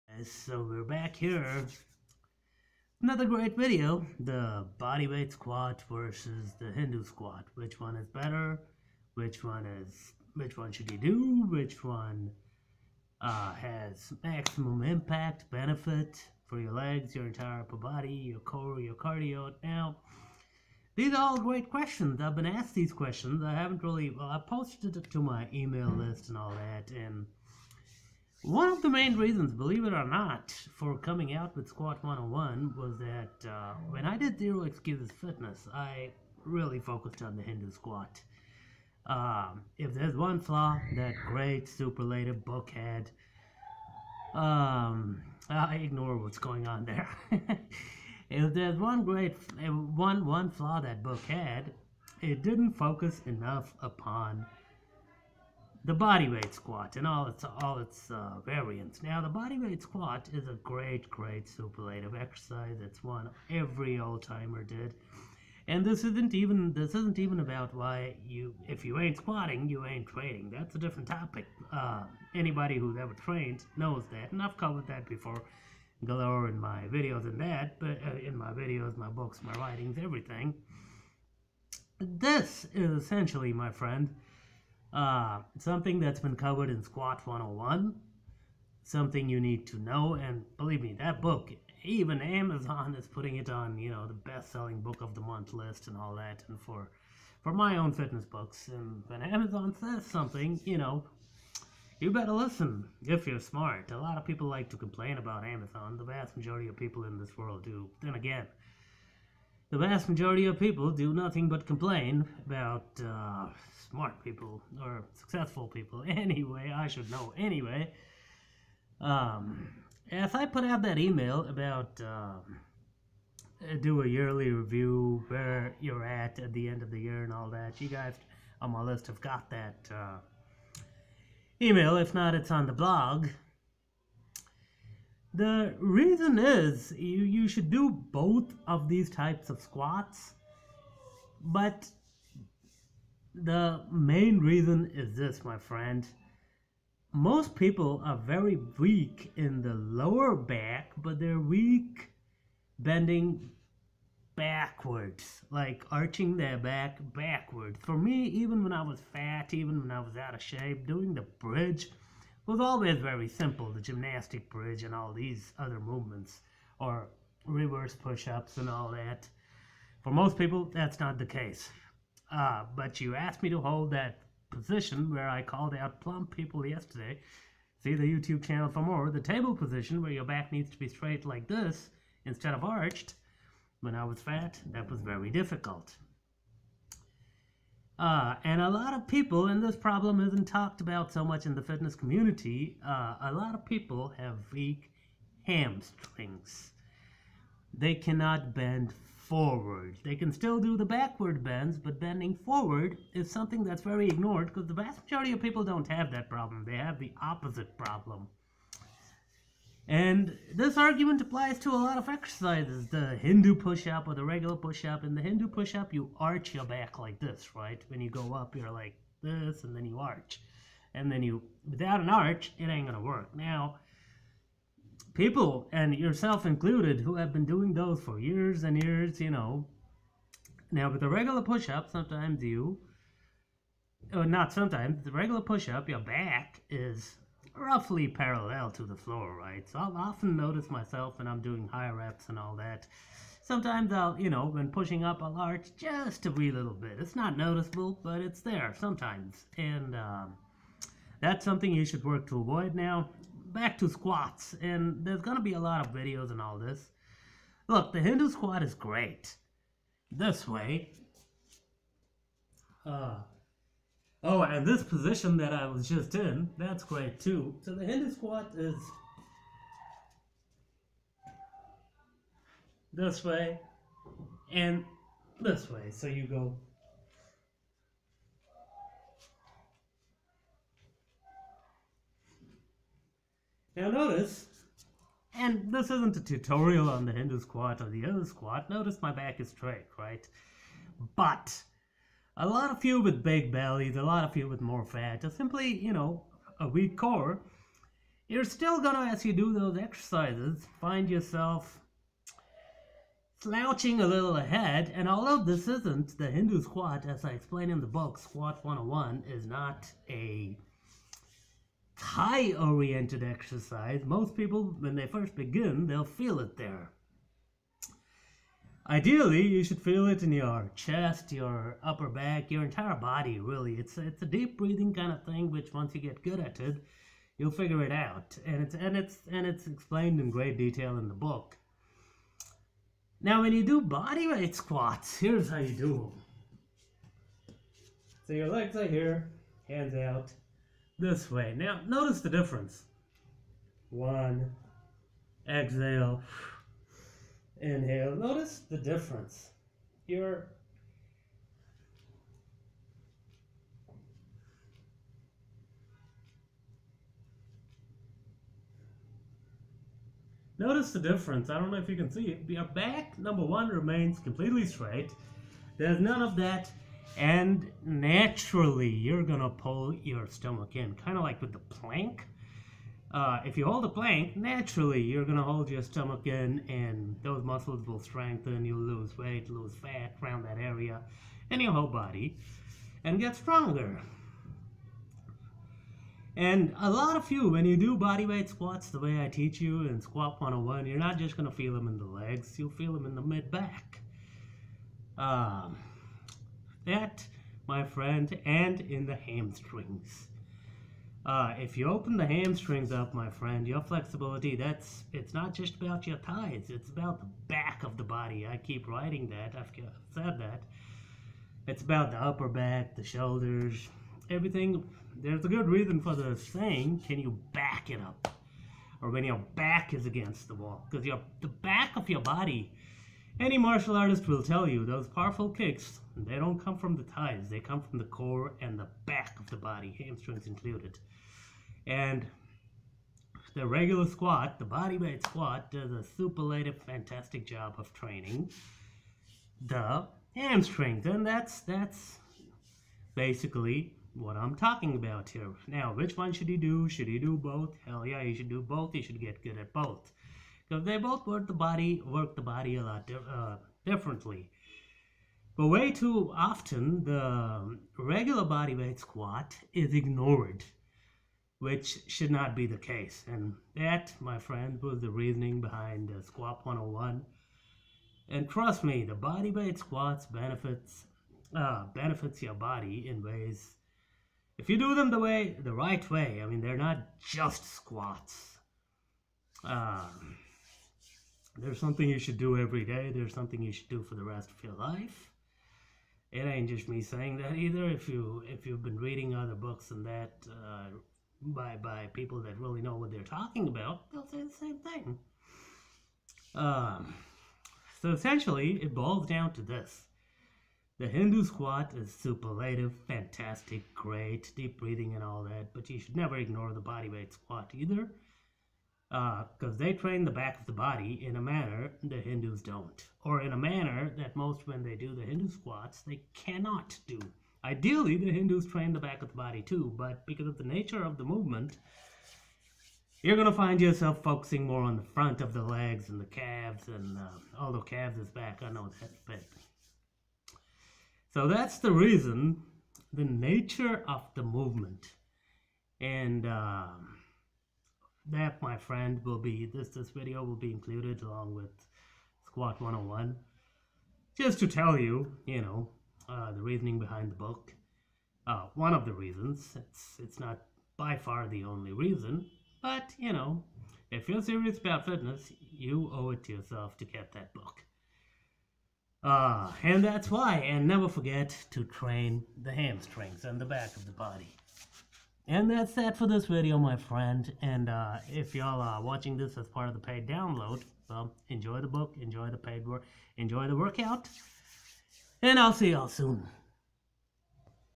Bodyweight squats vs Hindu Squats "101"!
Fancier exercises like the Hindu pushups and Hindu Squats get th due under the sun - rightfully so. they have immense benefits when done right.

but, one of the main reasons I wrote Squat 101?
was that the regular squat, unfortunately NEVER gets its due.

for lots of people that may not be necessarily a bad thing.

for those with naturally weak hamstrings and such though?

for those whom the back bridge is often easier than the front?

yes, these people exist .

they aren't really talked about a lot either, and that's fine - the other group is in the majority.

anyway.

and learn!

this has been sent out as an email too...

cheers
Rahul